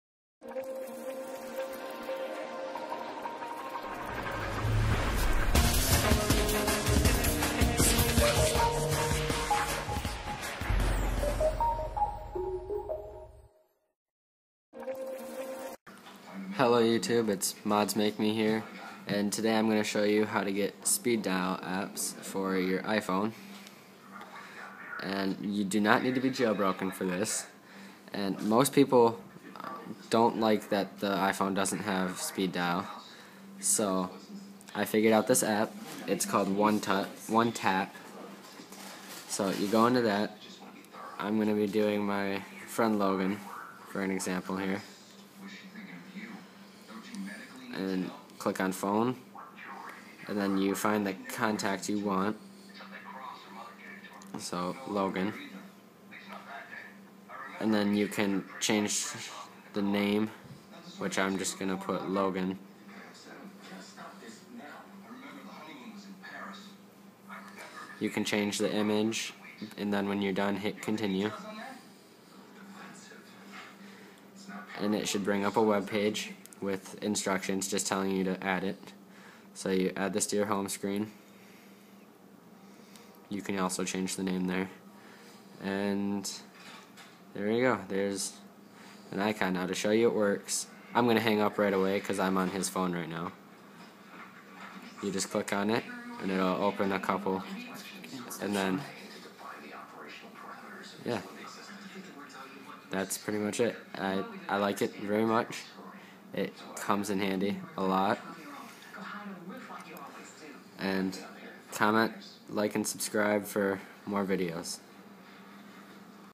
How to get speed Dial on Iphone
Hey guys, just a short tutorial on how to get speed dial icons on your springboard for iphone. If you want to know i got my apps set up i will have that video up shortly